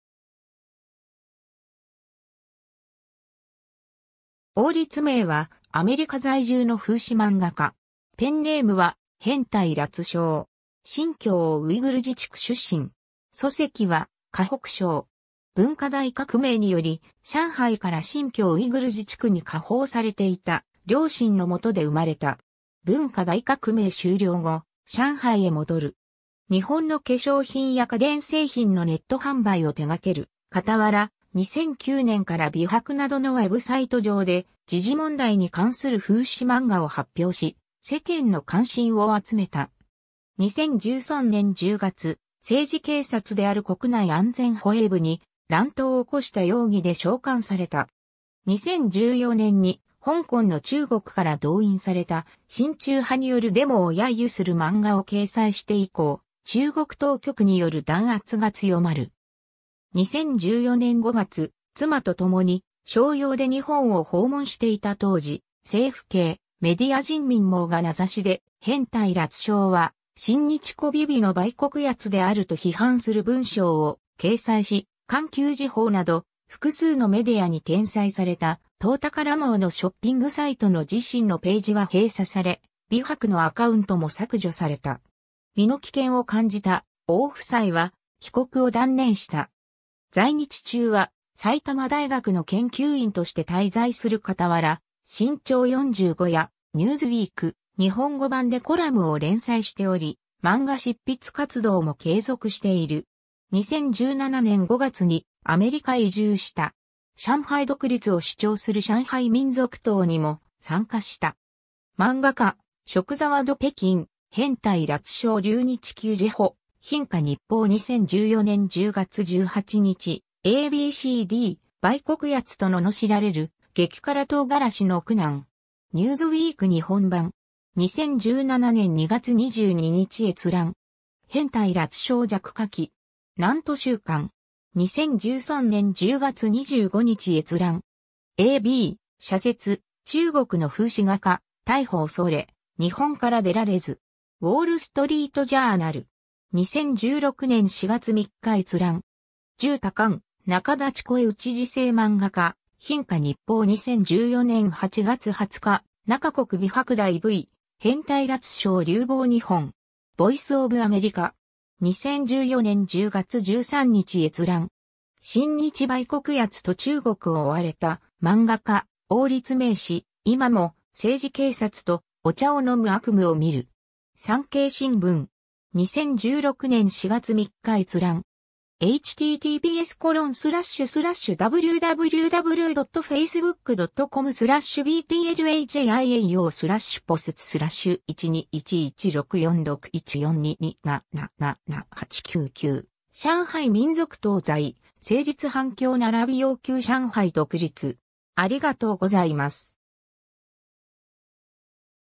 王立銘
王立銘（おう りつめい、1973年 - ）は、アメリカ在住の風刺漫画家。
ペンネームは「變態辣椒」（ビエンタイラージャオ、激辛唐辛子の意味）。
新疆ウイグル自治区出身。
 祖籍は河北省。
文化大革命により上海から新疆ウイグル自治区に下放されていた両親のもとで生まれた。
文化大革命終了後、上海へ戻る。
 日本の化粧品や家電製品のネット販売を手掛ける傍ら、2009年から微博などのウェブサイト上で時事問題に関する風刺漫画を発表し、世間の関心を集めた。
2013年10月、政治警察である国内安全保衛部（国保）に「乱闘を起こした容疑」で召喚された。
2014年に香港の中国から動員された親中派によるデモを揶揄する漫画を掲載して以降、中国当局による弾圧が強まる。
 2014年5月、妻とともに商用で日本を訪問していた当時、政府系メディア人民網が名指しで「變態辣椒は親日媚日の売国奴」であると批判する文章を掲載し、環球時報など複数のメディアに転載された、淘宝網のショッピングサイトの自身のページは閉鎖され、微博のアカウントも削除された。
身の危険を感じた王夫妻は帰国を断念した。
 在日中は埼玉大学の研究員として滞在する傍ら、新潮45やニューズウィーク日本語版でコラムを連載しており、漫画執筆活動も継続している。
2017年5月にアメリカへ移住した。
 上海独立を主張する上海民族党にも参加した。
 ^ 漫畫家觸怒北京 「變態辣椒」留日求自保「蘋果日報」2014年10月18日 ^ a b c d “売国奴と罵られる「激辛トウガラシ」の苦難 (1/3)”. ニューズウィーク日本版 (2014年12月2日). 2017年2月22日閲覧。
 ^ “「變態辣椒」惹禍記” (簡体字). 南都週刊. 2013年10月25日閲覧。
 ^ a b “【社説】中国の風刺画家、逮捕恐れ日本から出られず”. ウォールストリートジャーナル. 2016年4月3日閲覧。
 ^ 十多官媒聲討時政漫畫家「蘋果日報」2014年08月20日 ^ “中國微博大V「變態辣椒」流亡日本” (中国語). ボイス・オブ・アメリカ. 2014年10月13日閲覧。
 ^ “「親日売国奴」と中国を追われた漫画家・王立銘氏「今も政治警察と『お茶を飲む』悪夢を見る…」”. 産経新聞. 2016年4月3日閲覧。
^ 「上海民族党」在纽约成立 反共并要求上海独立